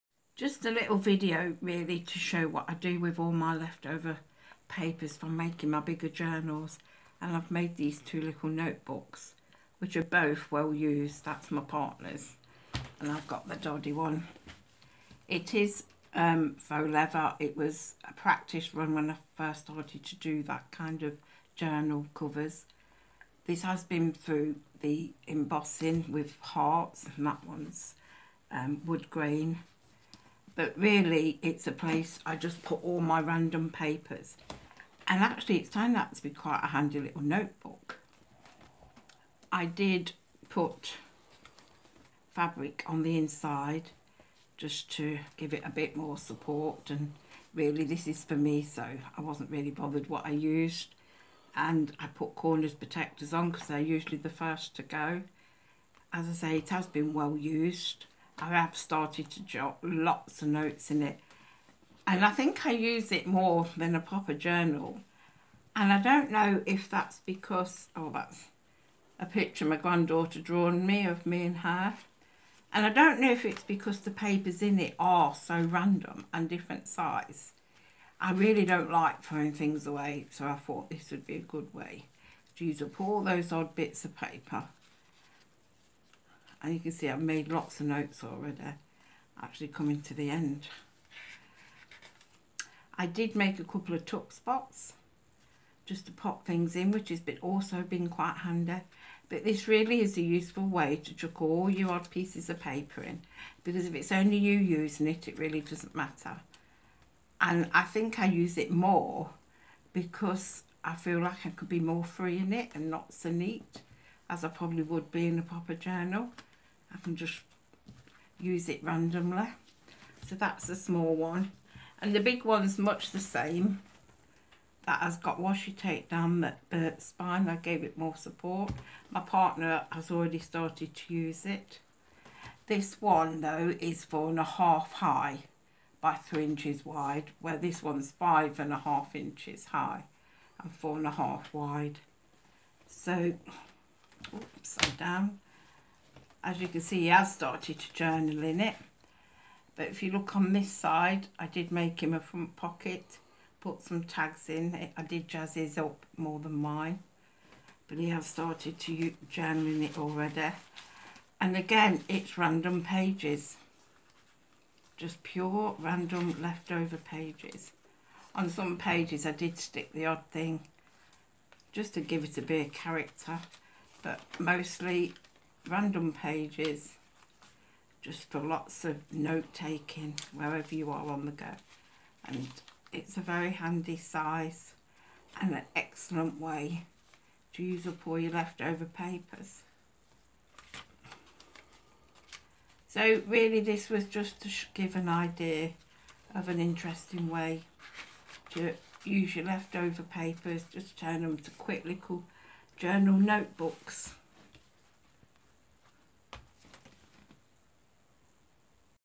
Journal mini,
Two small journals to use up all my left over paper. Handy to have to not notes in.